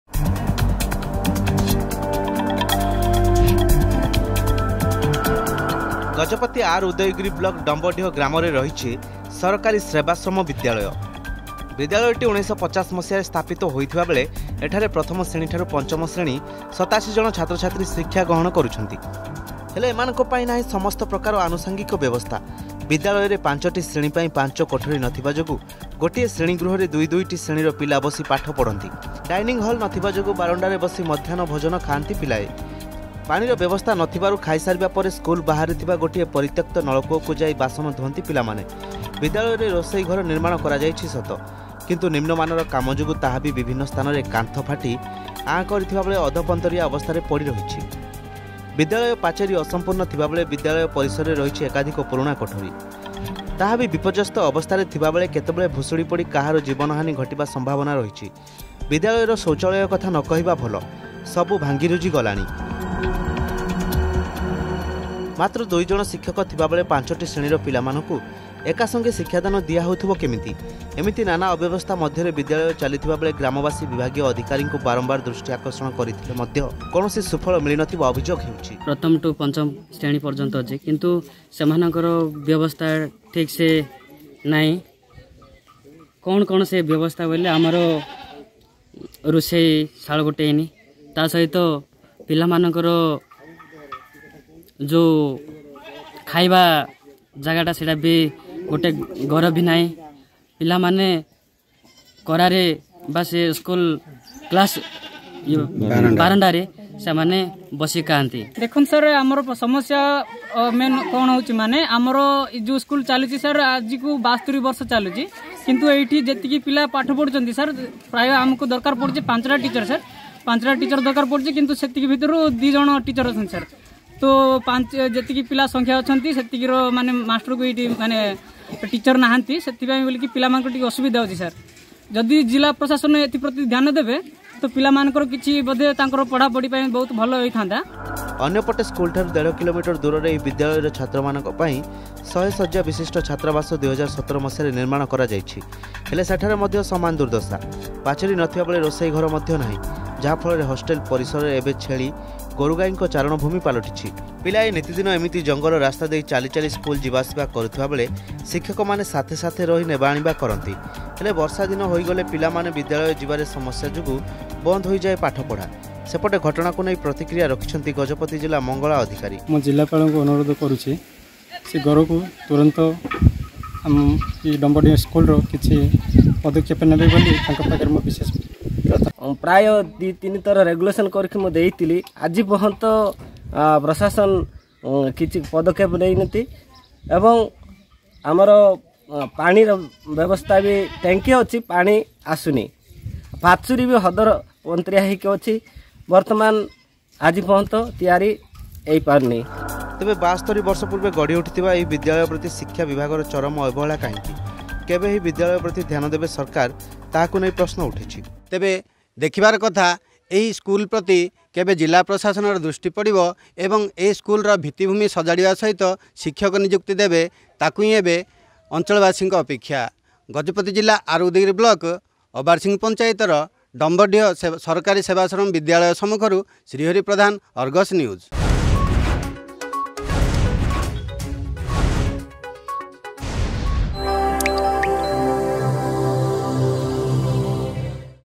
ଛାତ୍ର ଛାତ୍ରୀଙ୍କ ଉଜ୍ଜ୍ବଳ ଭବିଷ୍ୟତ ପାଇଁ ନାହିଁ ଭଲ ସ୍କୁଲଟିଏ
ଉନ୍ନତ ମାନର ଶିକ୍ଷା ଦେବା ସହ ବିଦ୍ୟାଳୟର ଭିତିଭୂମି ସଜାଡିବା ପାଇଁ ସରକାର ବିଭିନ୍ନ ଯୋଜନା ମାଧ୍ୟମରେରେ ଖର୍ଚ୍ଚ କରୁଛନ୍ତି କୋଟି କୋଟି ଟଙ୍କା । ହେଲେ ବିଭାଗୀୟ ଅଧିକାରୀଙ୍କ ଇଛାଶକ୍ତିର ଅଭାବ, ଠିକାଦାରଙ୍କ ମଧୁଚନ୍ଦ୍ରିକା ଯୋଗୁଁ ସବୁ ସରକାରୀ ଯୋଜନା ଏବେ ବାଟହୁଡିବାରେ ଲାଗିଛି ! ଗଜପତି  ଆର୍ ଉଦୟଗିରି ବ୍ଲକ୍ ଡମ୍ବଡିହ ଗ୍ରାମରେ ରହିଛି ସରକାରୀ ସେବାଶ୍ରମ ବିଦ୍ୟାଳୟ ।